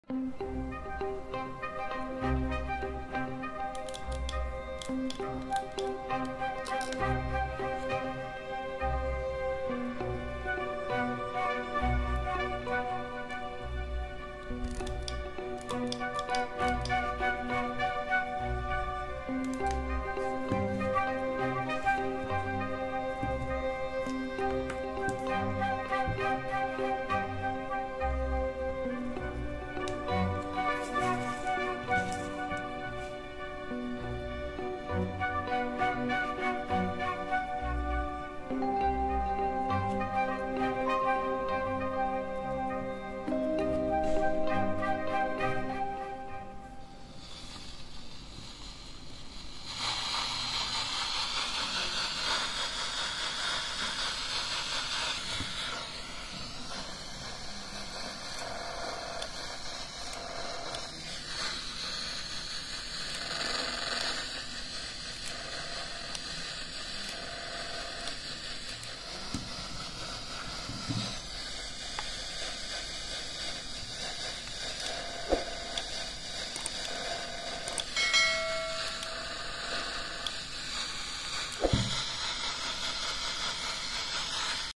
How to test Car Remote | Easy and working method !
How to check car remote control with your phone. Super easy and helpfull trick that everyone should know before visiting locksmith.